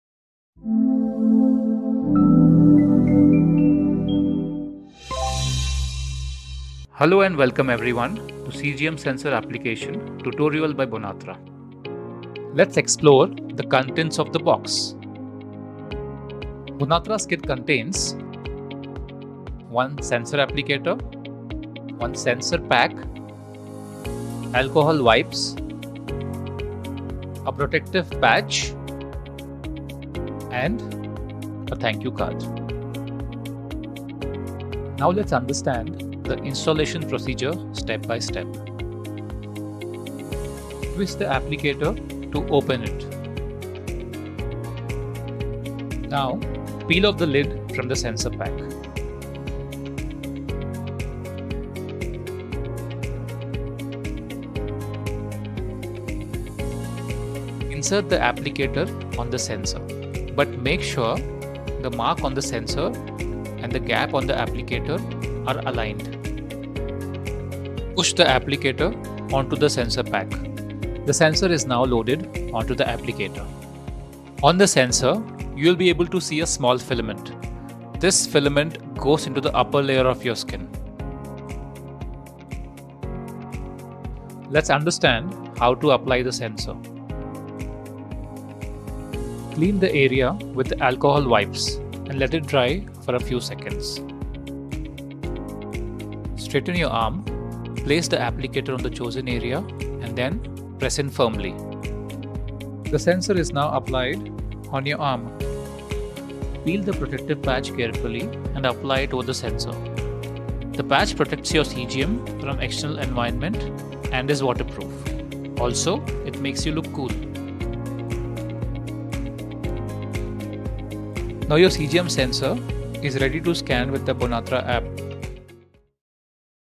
Continuous Glucose Monitor Installation Steps | Bonatra
Bonatra's kit is thoughtfully designed, containing all you need for a seamless installation: a Sensor Applicator, Sensor Pack, Alcohol wipes, a Protective Patch, and even a heartfelt Thank You card. The installation process is a breeze:

1. Twist the applicator to open it.
2. Carefully peel off the lid from the Sensor Pack.
3. Insert the applicator onto the sensor, ensuring that the mark on the sensor and the gap on the applicator align perfectly.
4. Push the applicator onto the sensor pack; this loads the sensor onto the applicator.
5. Prior to application, cleanse the chosen area with an alcohol wipe, allowing it to dry for a few seconds.
6. Straighten your arm and place the applicator on the designated spot, pressing it firmly.
7. The sensor is now expertly applied to your arm. Gently peel off the protective patch and securely attach it to the sensor.

Your Bonatra CGM is now comfortably in place, ready to support your health journey.